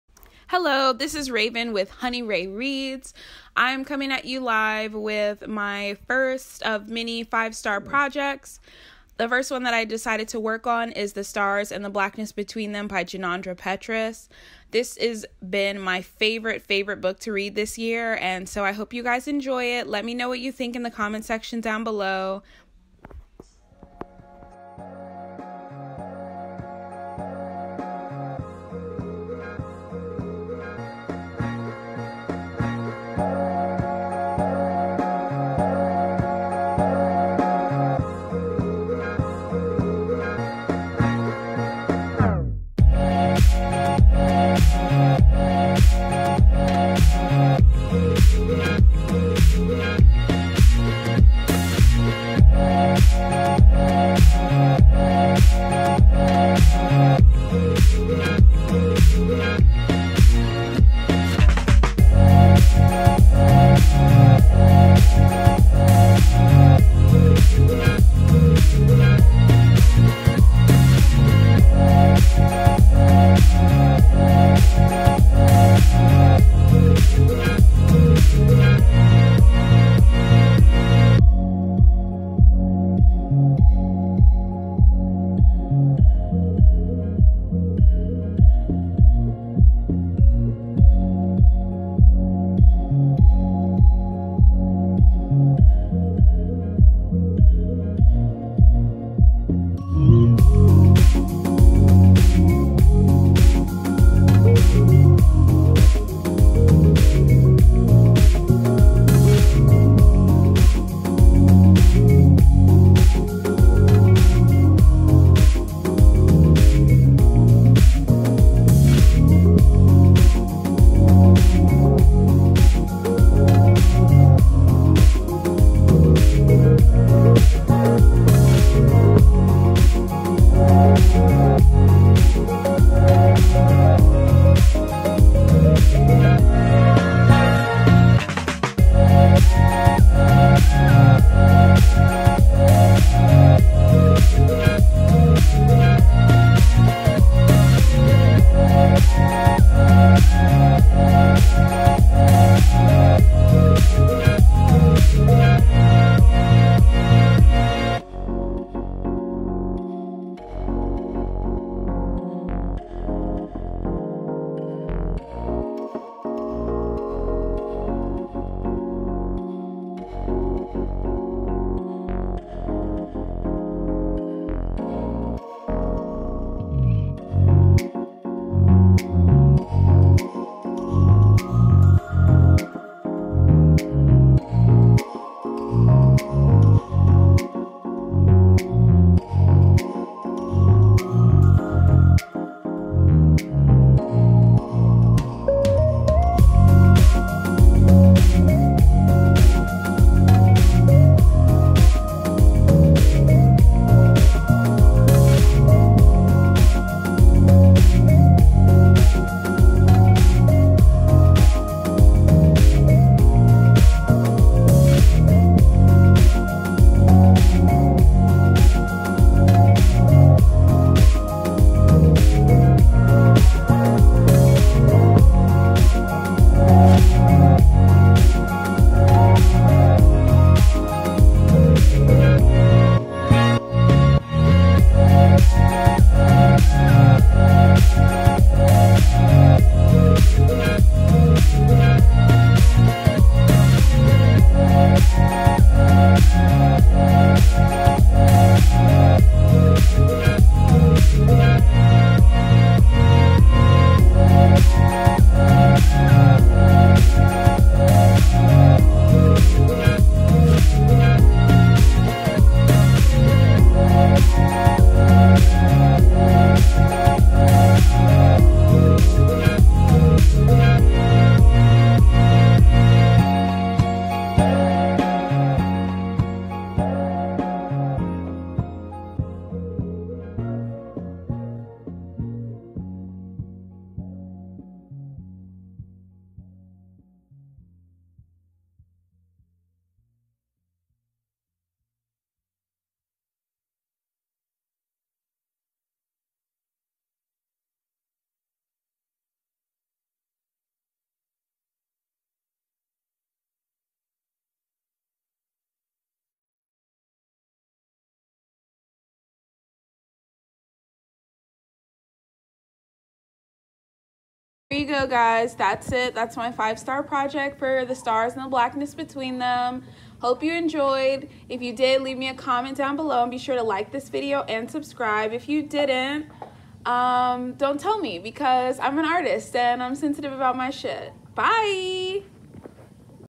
Five Star Projects: The Stars and the Blackness Between Them
For every one of my five star reads, I'm doing a special themed project! This one is a painting for my favorite read for July, The Stars and the Blackness Between Them by Junauda Petrus! I love doing art and journaling for books! If you have any ideas for future projects drop them in the comments below!